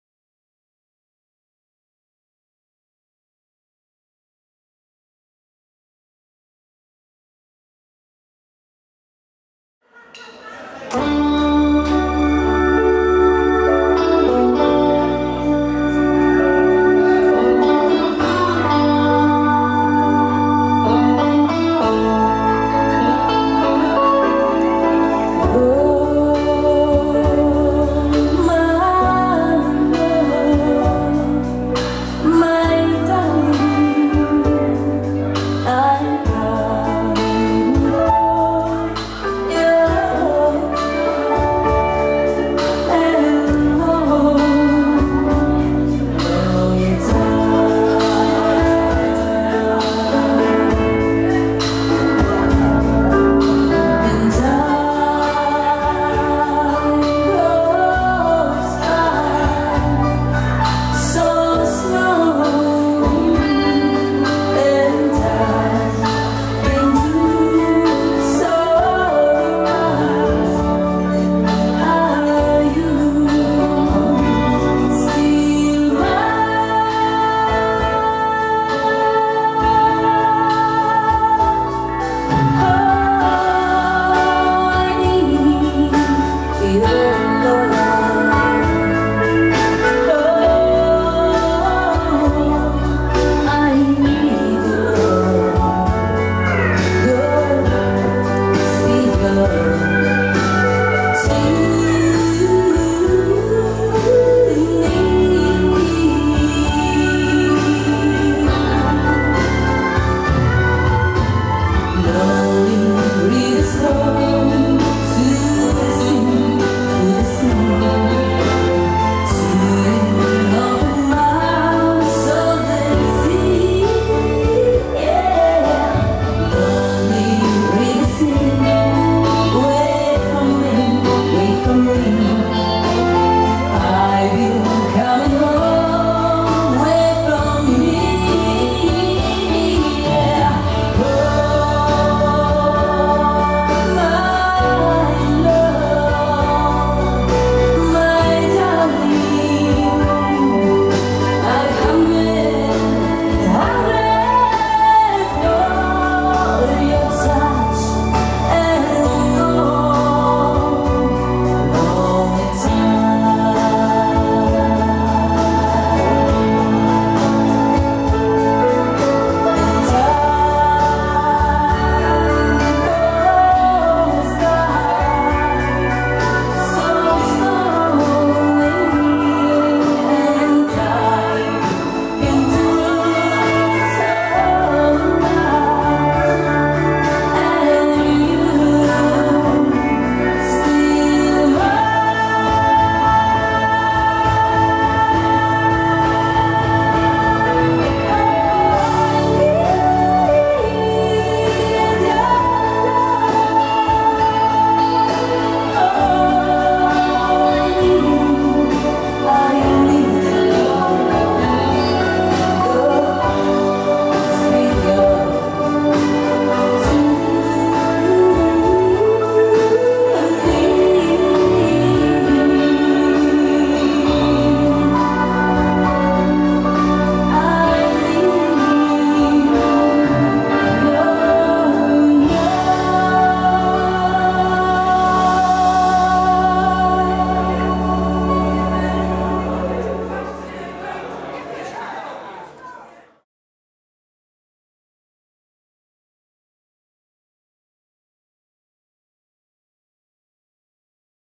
Zespół AGAT i NPICTURES klip weselny
Klip zrealizowany na weselu Gosi i Szymona